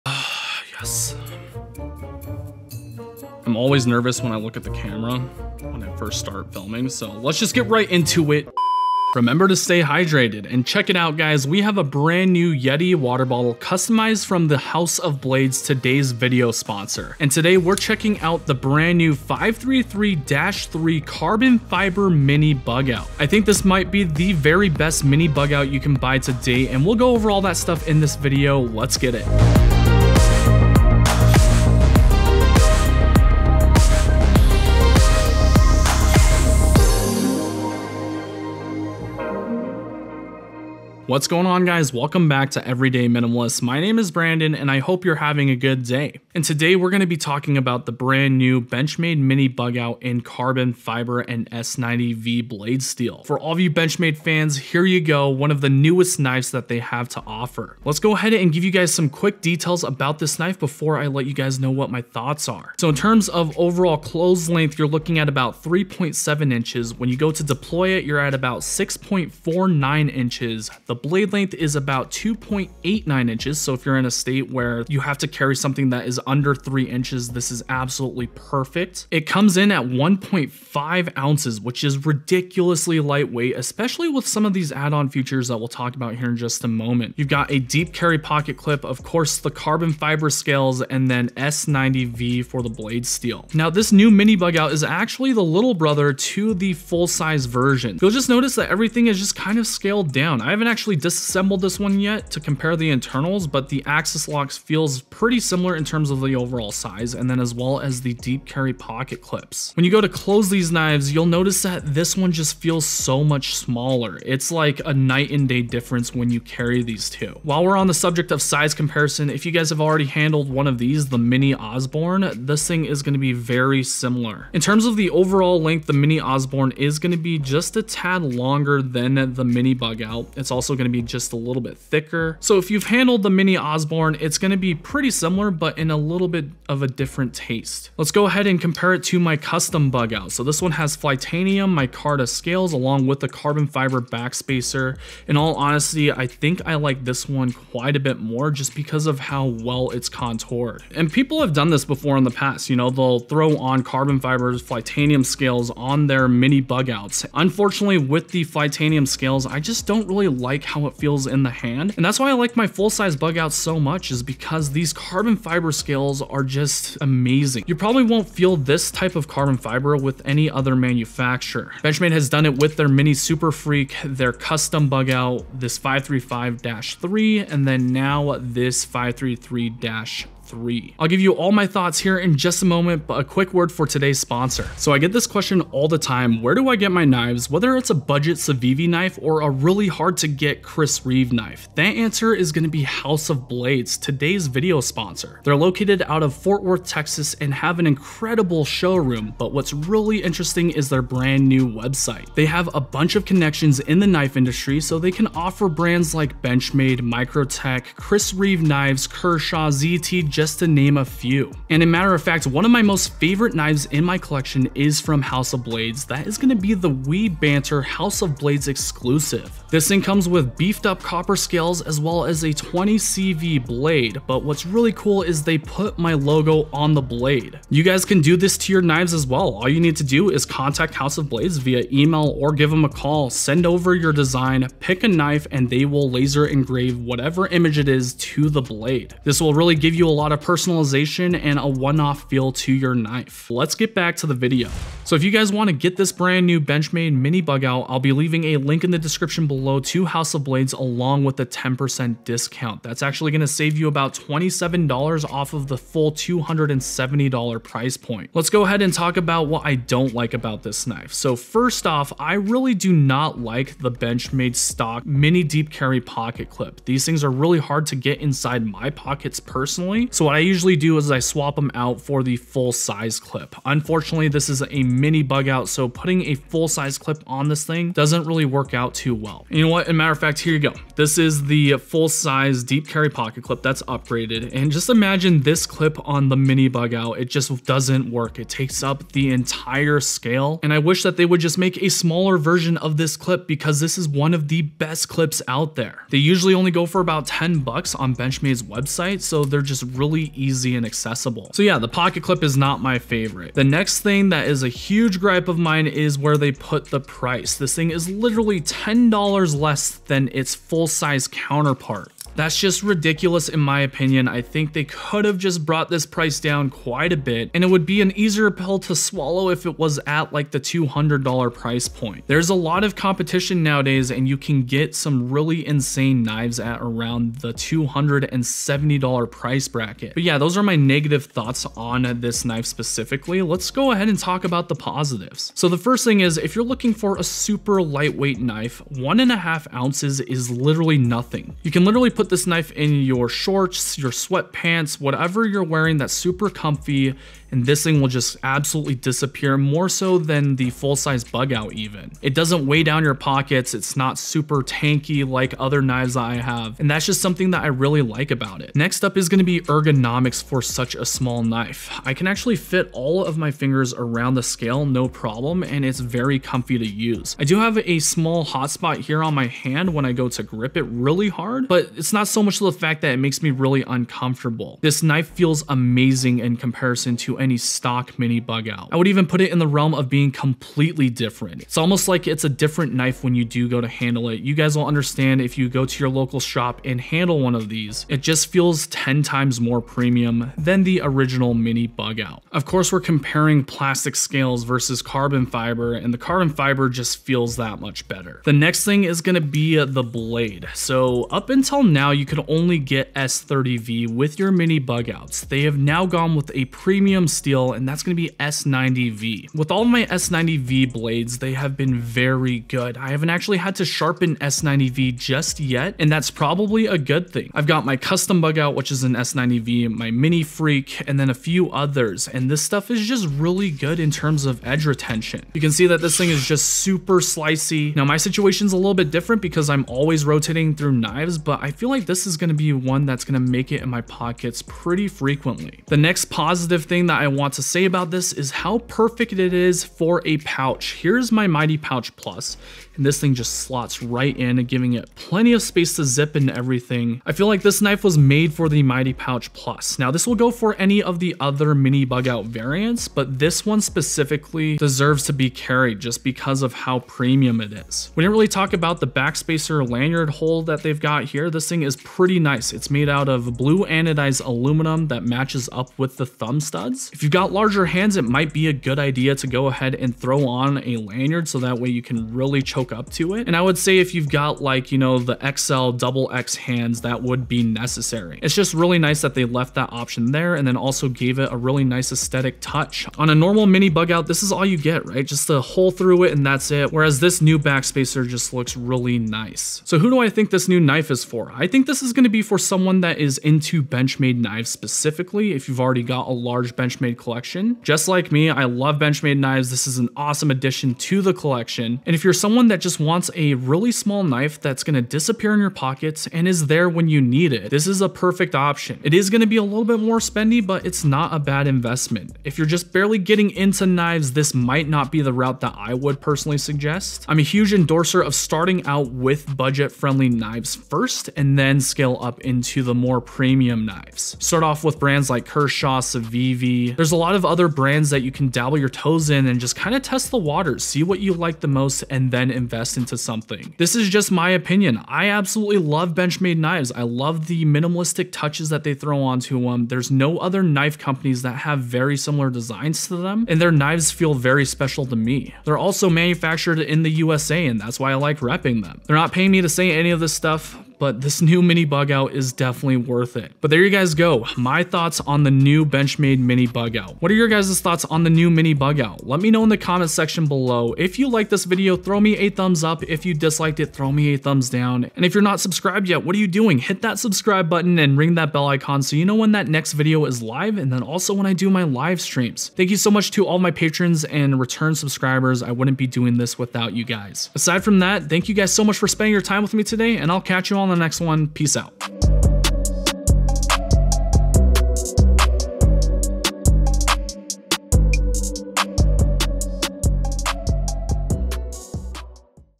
The BEST Mini Bugout? | Benchmade Mini Bugout 533-3 (Carbon Fiber+S90V)
Today we're checking out the NEW MINI BUGOUT. This is probably one of the best mini folding knives from Benchmade. It's got carbon fiber handles, premium S90V blade steel and a few other features. There are some negatives about this knife which we'll discuss in this video!